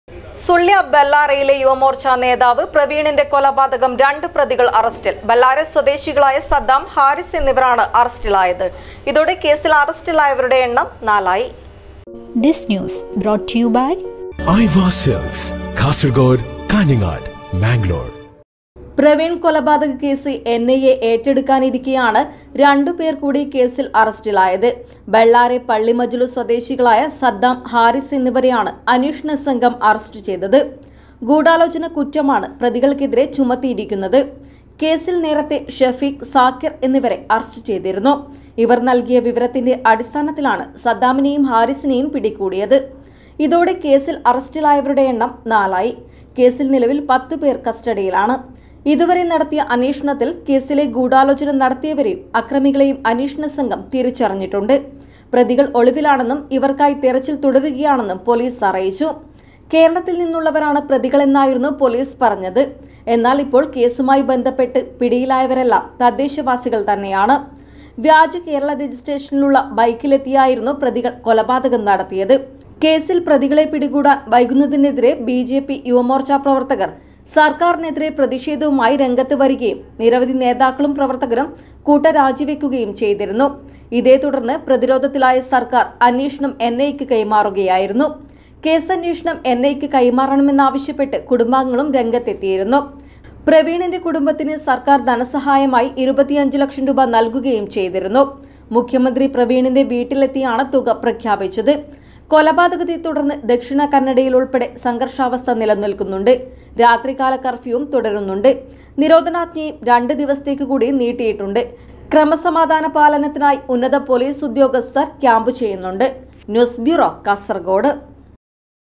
സുള്ള്യ ബെല്ലാരെയിലെ യുവമോര്‍ച്ച നേതാവ് പ്രവീണിന്റെ കൊലപാതകം രണ്ട് പ്രതികള്‍ അറസ്റ്റില്‍
സുള്ള്യ ബെല്ലാരെയിലെ യുവമോര്‍ച്ച നേതാവ് പ്രവീണിന്റെ കൊലപാതകം രണ്ട് പ്രതികള്‍ അറസ്റ്റില്‍. ബെല്ലാരെ സ്വദേശികളായ സദ്ദാം,ഹാരിസ് എന്നിവരാണ് അരസ്റ്റിലായത്.